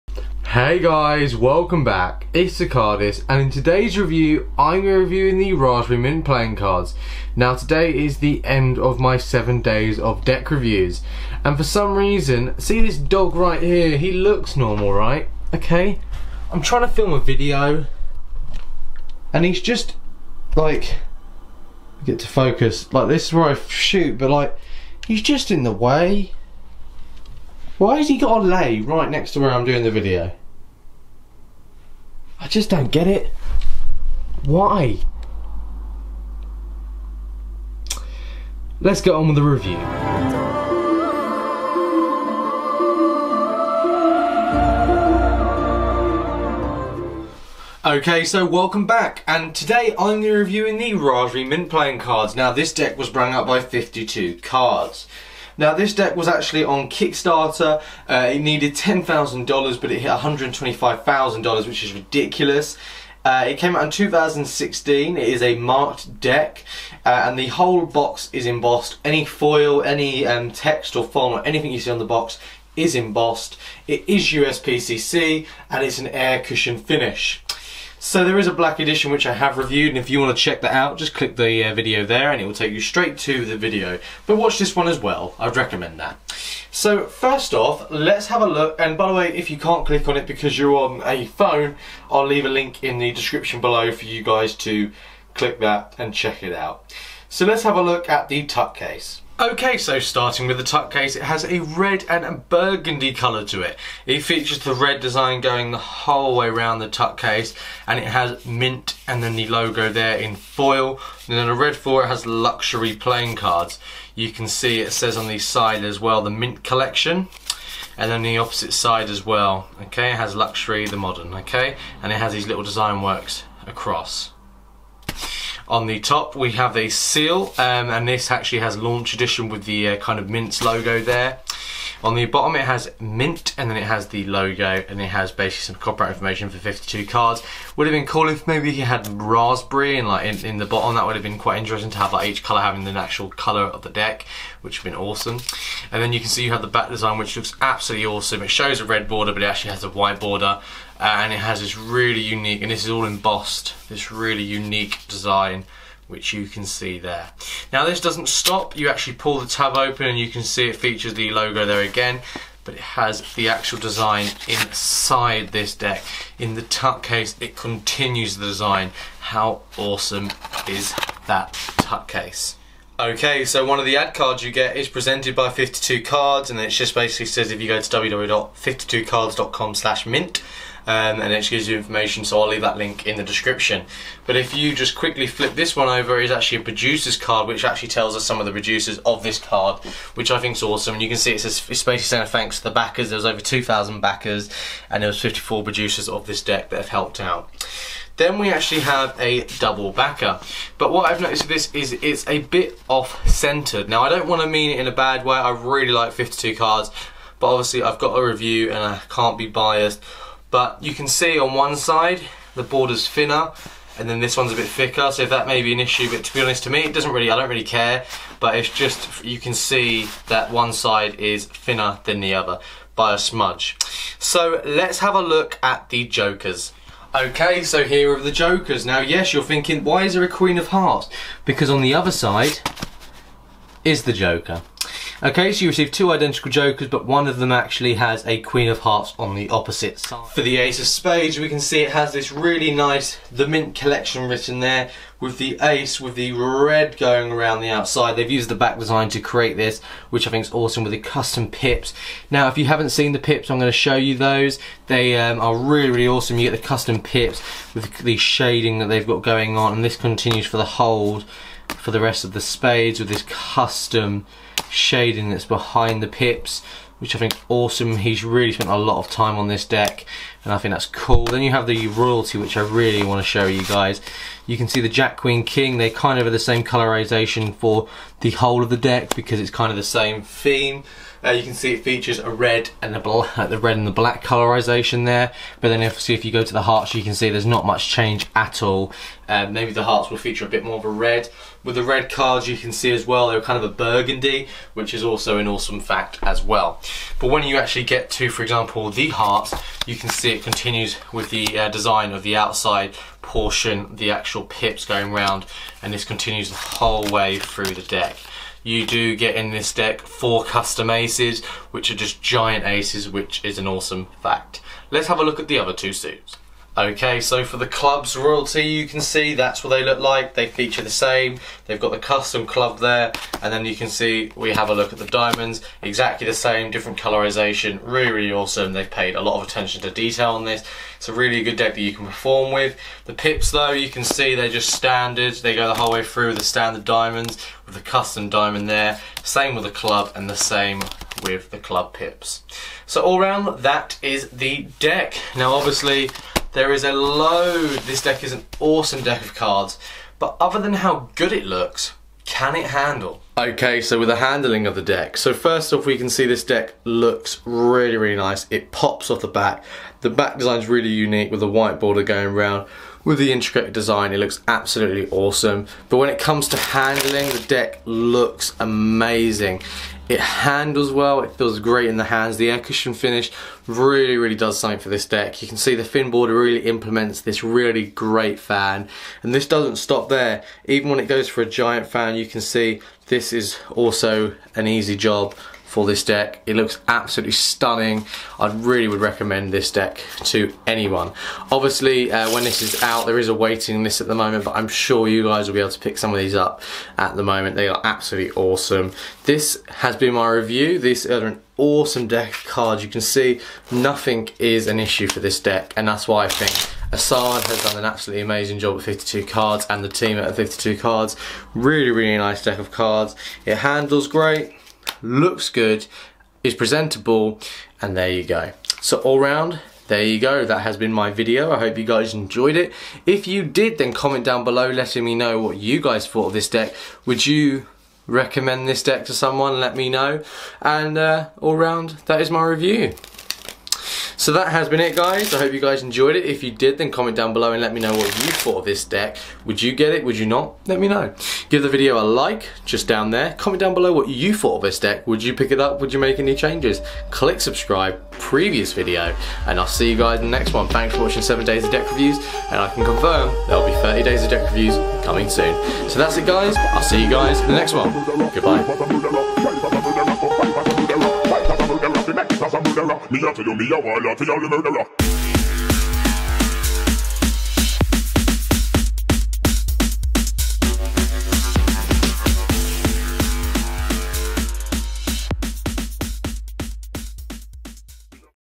Deck Review - Raspberry Mint Playing Cards By 52Kards [HD-4K]
- Producers of this deck - Albino Dragon Kickstarter project -
We wanted to create a worker's deck. A pack of 52 cards that merges form with function. Using a classical approach to design, the goal was to develop a look that is familiar, but fresh. The elegant repeating pattern evokes the decks that have survived the test of time. Black Mint Playing Cards are luxury for the modern cardician.

- TheCardists Teams Thoughts.
- Black Mint Playing Cards By 52Kards was funded on Kickstarter and this is how the deck was brought to life! this is a unique deck of cards and I love the feature of the joker and a queen it works well for a trick I show in the video.

● Handling - 8/10
● Tuck-box - 7.5/10
● Overall Design - 7.5/10
● Overall Score - 7.5/10

If you have a minute to spare why not fill in my survey to make the channel better would be much appreciated